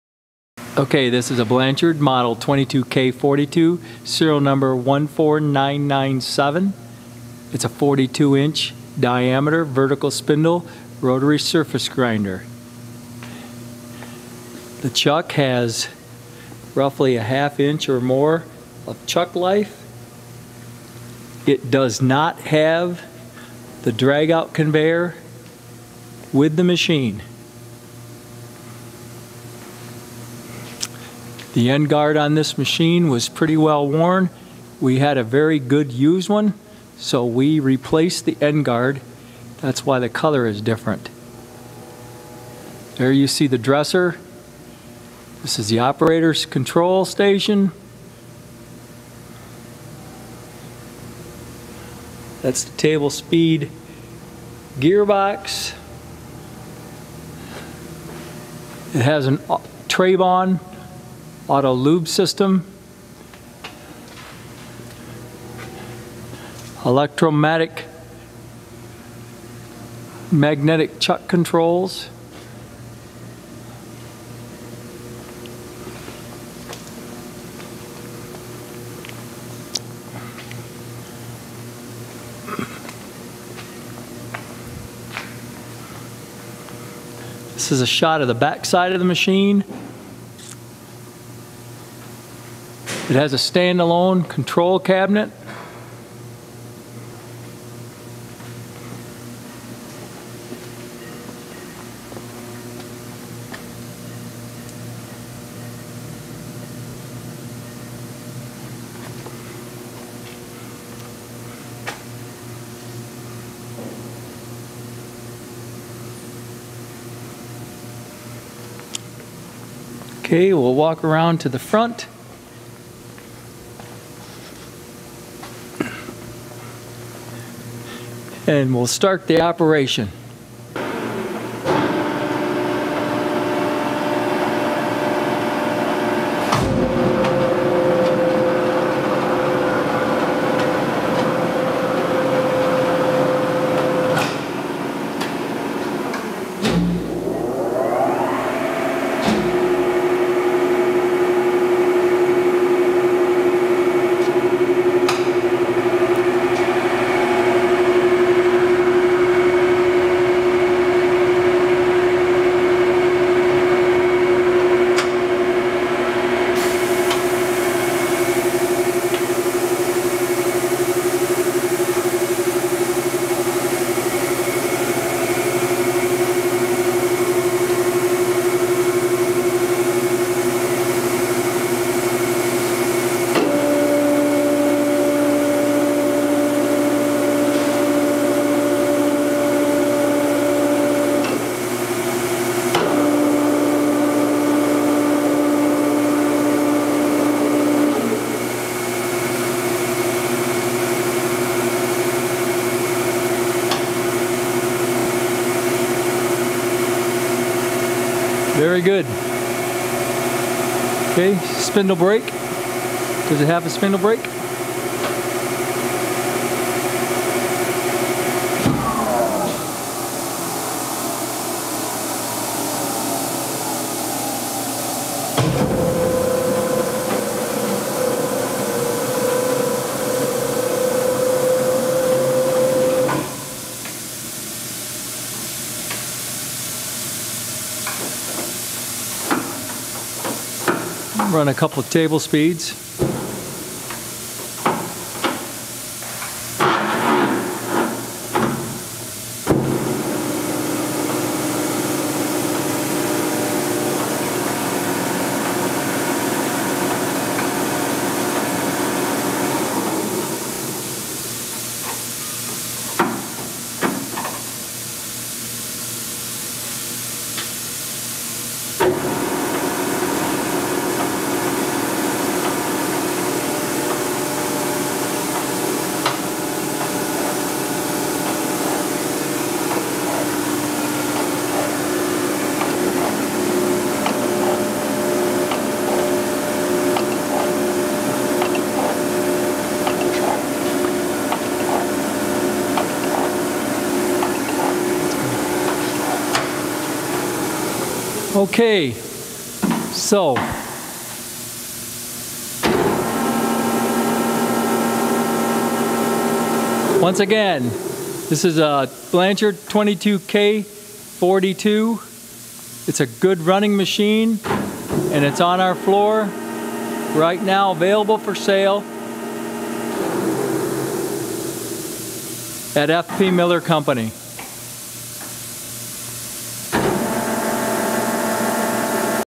Blanchard 22K-42, 42" Vertical Spindle Rotary Surface Grinder
Tag Number: 16592
Machine Type: Grinding Machines
UMCC: G8100
Manufacturer: Blanchard
Model: 22K-42
Year: 1978
Serial Number: 14997
Description: Blanchard 22K-42, 42" Vertical Spindle Rotary Surface Grinder

Overall Specifications
Diameter of Magnetic Chuck: 42", 5/8" Chuck Life
Grinding Wheel Diameter: 22" Cortland Style Segmented Type
Maximum Distance Under New Wheel: 24"
Table Speeds (6): 6 - 33 RPM
Hand and Power Elevation to Wheel Head
Automatic Down Feed to Wheel Head: .004 - .080 IPM
Electrics: 480/3/60
Equipped with:
Hardened Bedways
Trabon Lubrication
Wheel Dresser
Coolant Pump in Base
Conveyor Base (Conveyor Not Included)
Machine has been tested and is in great operating condition